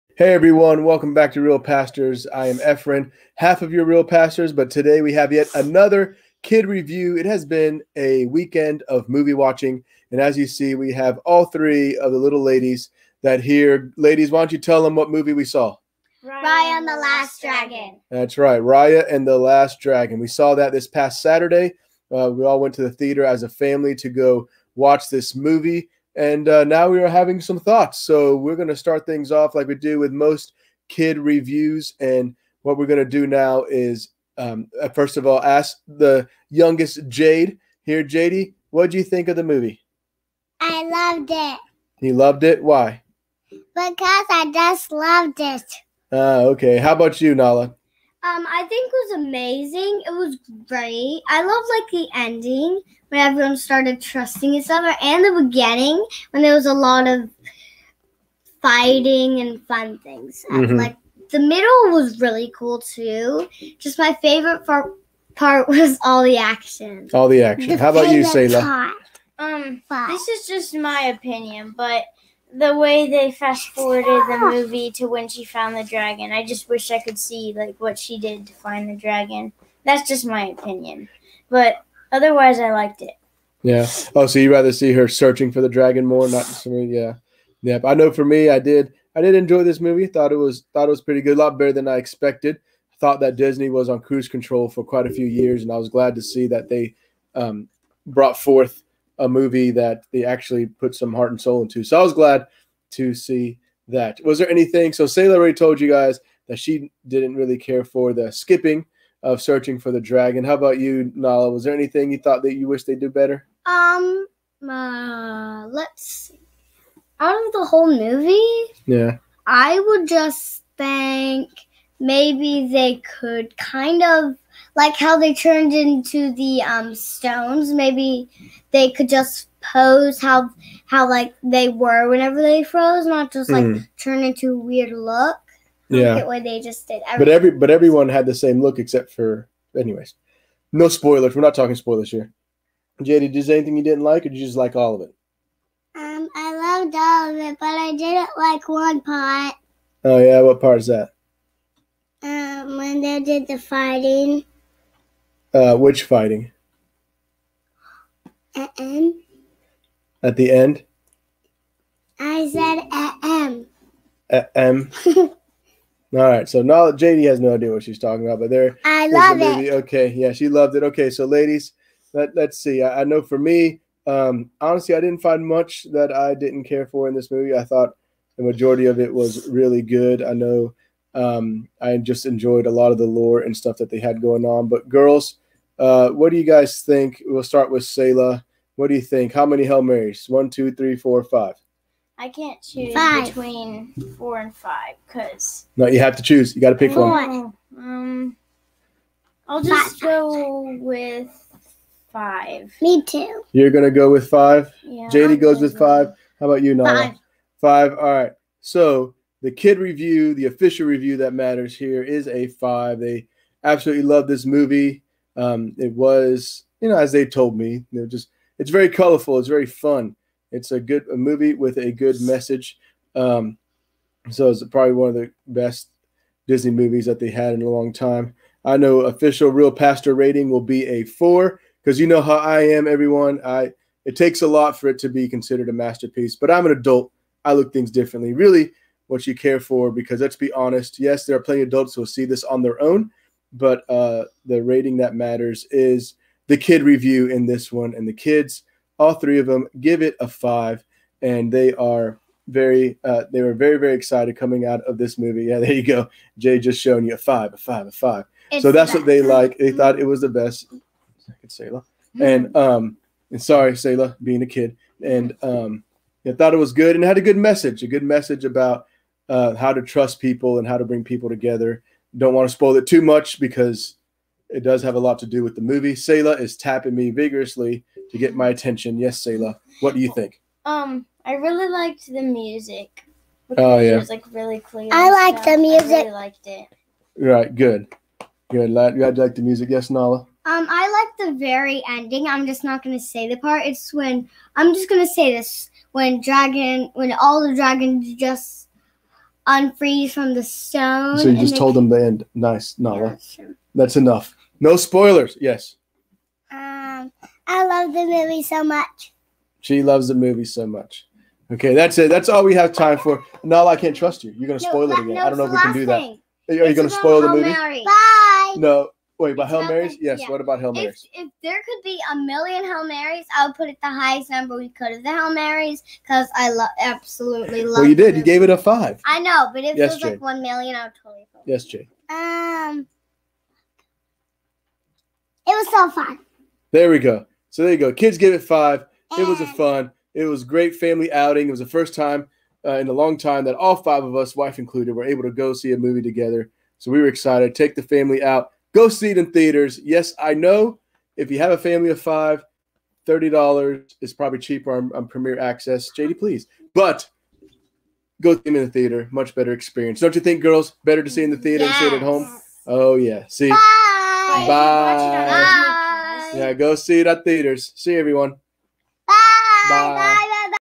Raya and the Last Dragon - Kid Review!
Check our our latest kid review! More kids more review!! See what they though of Raya and the Last Dragon and see how many hail Mary's they give it!